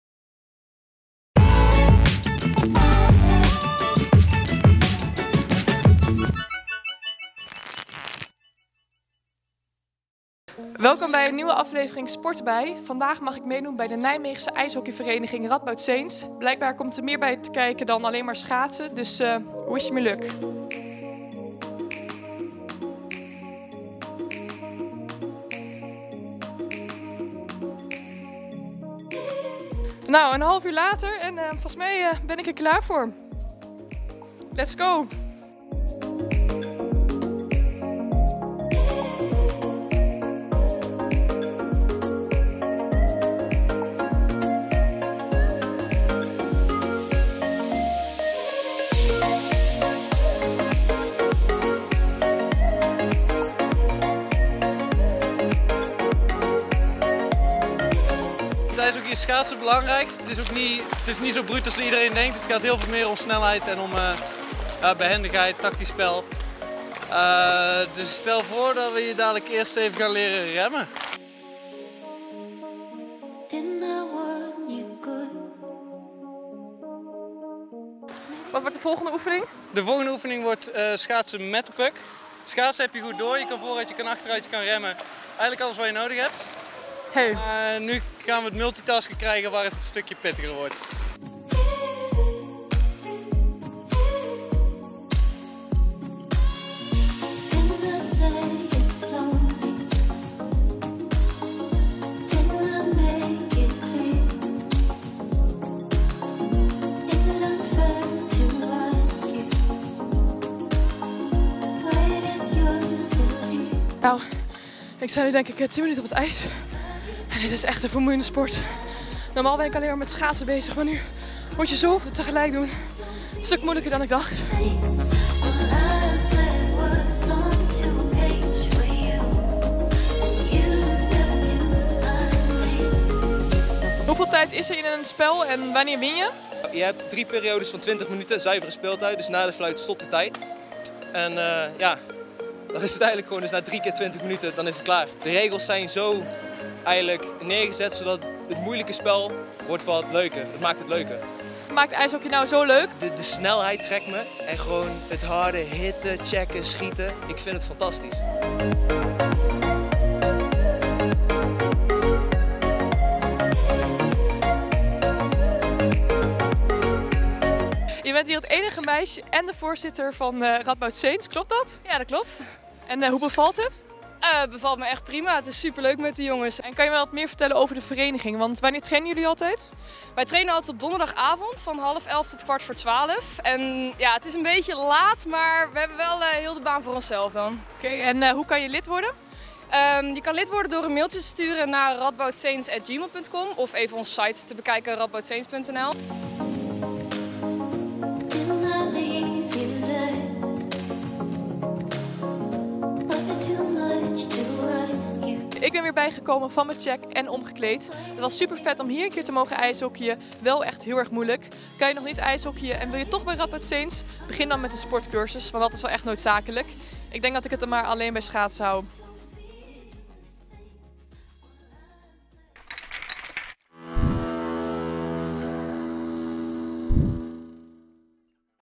Sporten bij: NSIJV Radboud Saints
In een nieuwe aflevering van 'Sporten bij' bezoekt Nienke de Nijmeegse IJshockeyvereniging Radboud Saints. Geheel in outfit, trekt ze ten strijde op de ijsbaan.